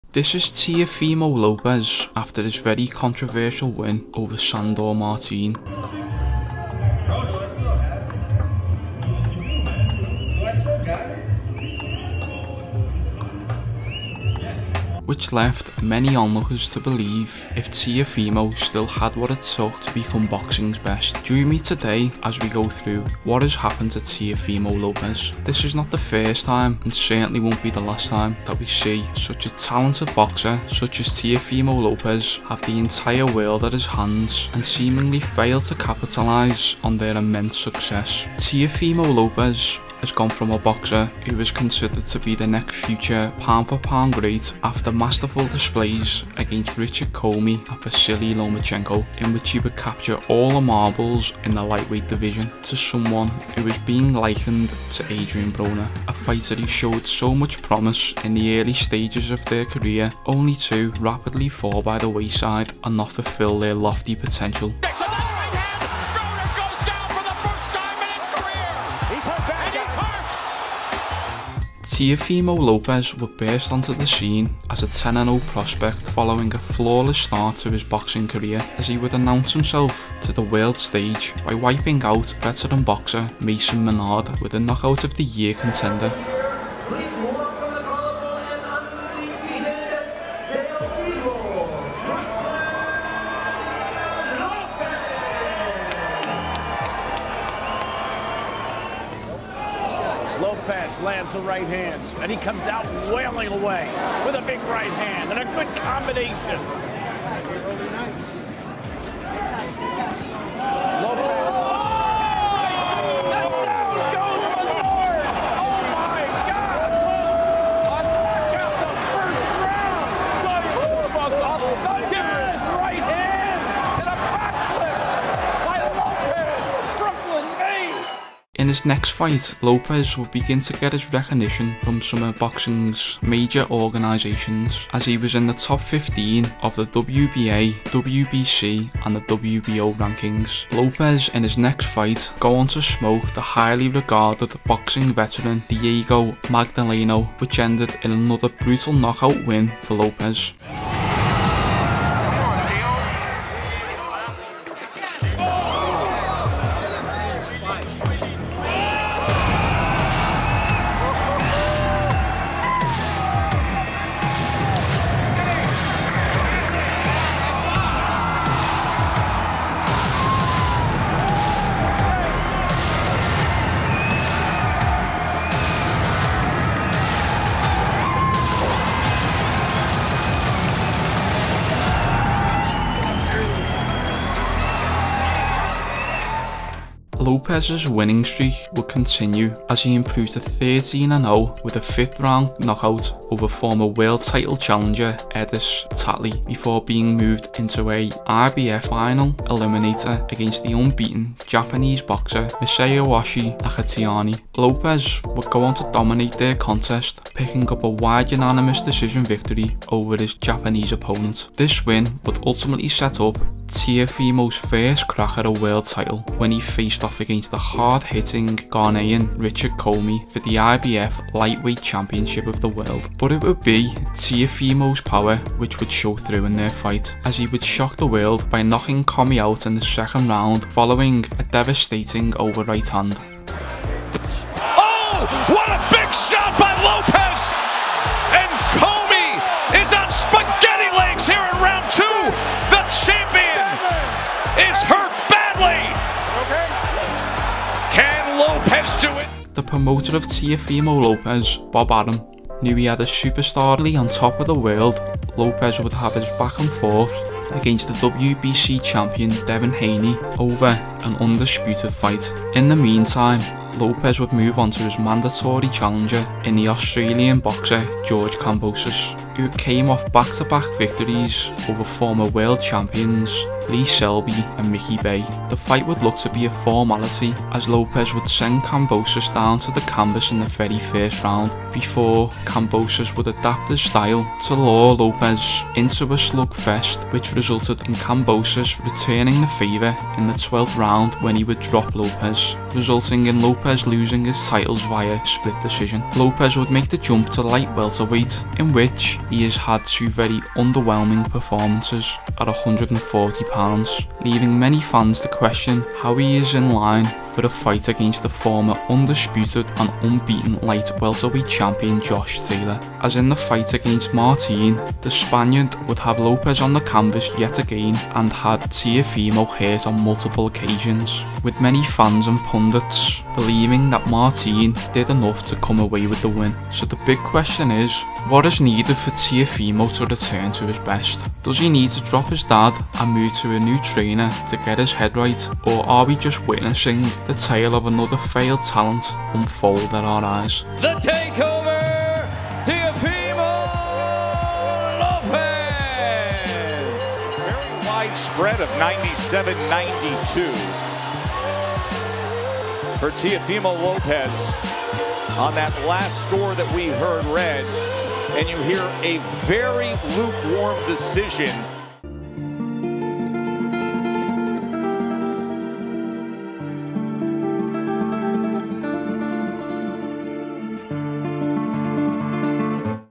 WTF HAPPENED To Teofimo Lopez?!
Here we will take a look back on the rise and fall of the former unified lightweight champion of the world Teofimo Lopez. A fighter who was viewed to be the "next big thing of boxing" all for him to become a fighter who has been completely written off by most pundits and fans.

0:00 Intro
1:19 Lopez vs Menard
2:08 Lopez vs Magdaleno
3:06 Lopez vs Tatli
3:15 Lopez vs Nakatani
3:36 Lopez vs Commey
4:10 Lopez vs Lomachenko
5:14 The Fall of Lopez